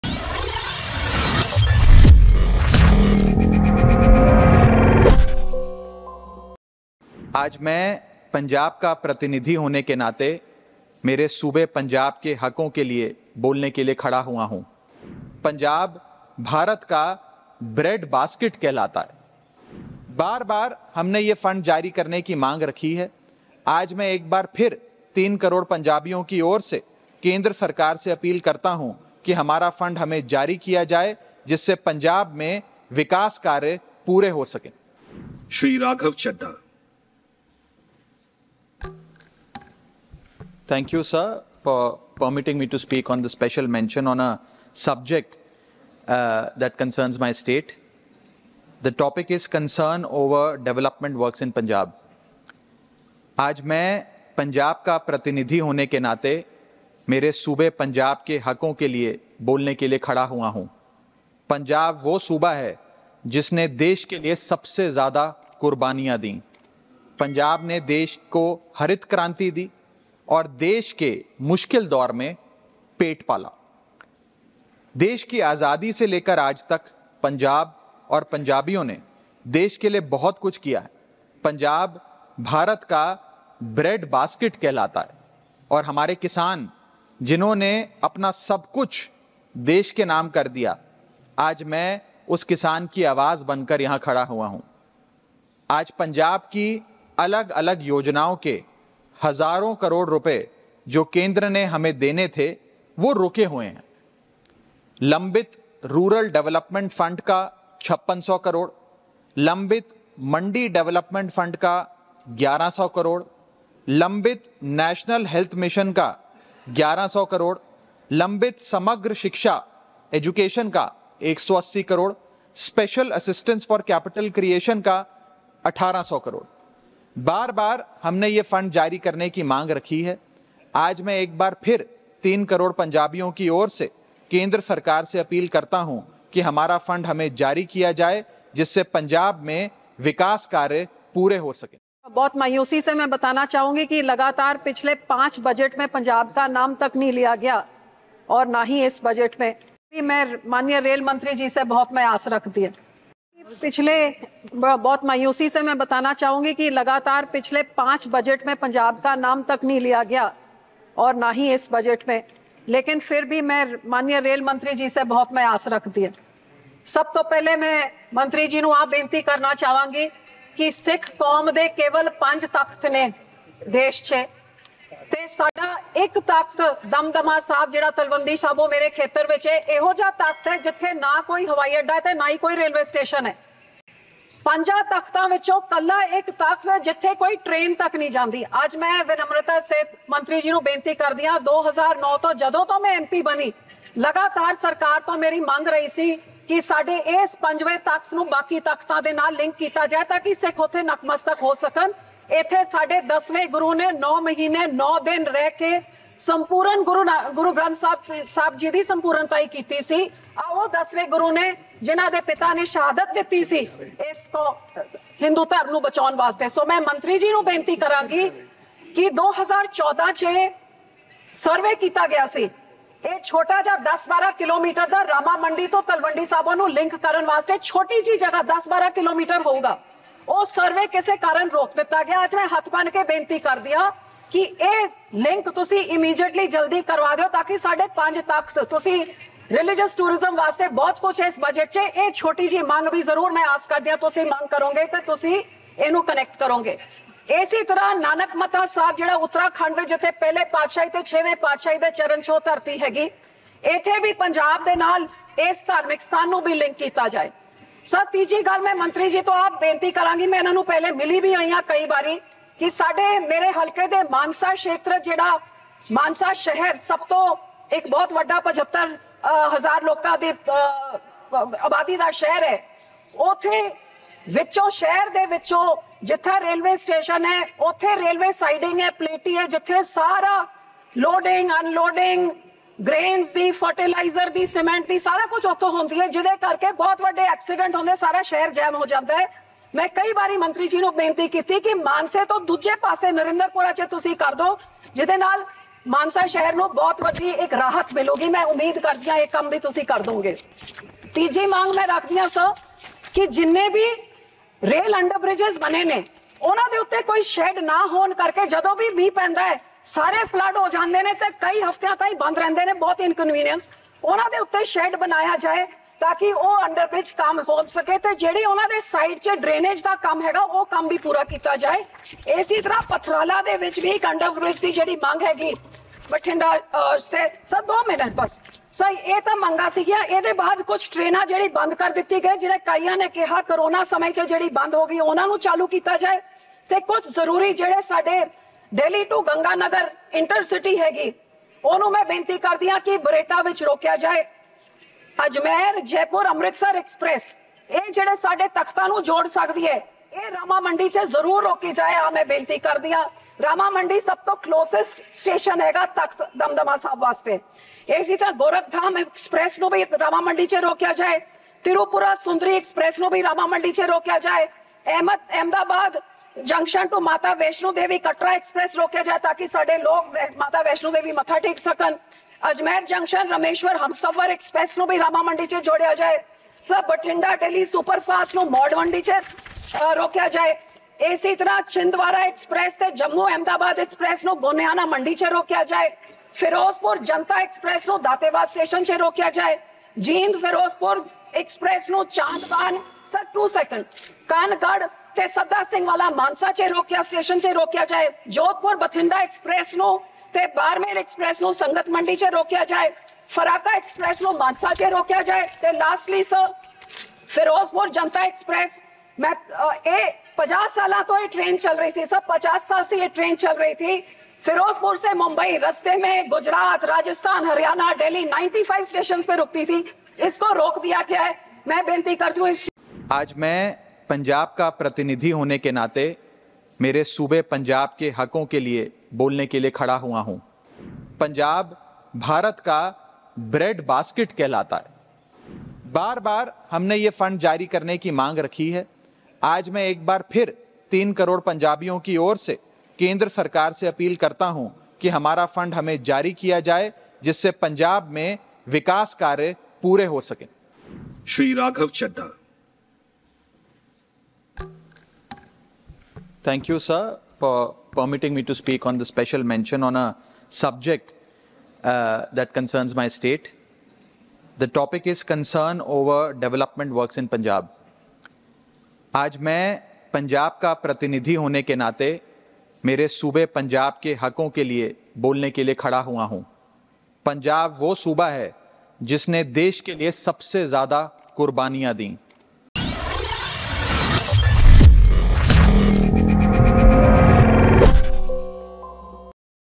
Raghav Chadha ने पंजाब के लिए PM Modi से की ऐसी मांग, सवालों में खुद ही फंसे !
संसद में आप सांसद राघव चड्ढा और हरसिमरत कौर ने पंजाब को लेकर मोदी सरकार  मांग की है

TANIYA SINGH
SHABNAM SAIFI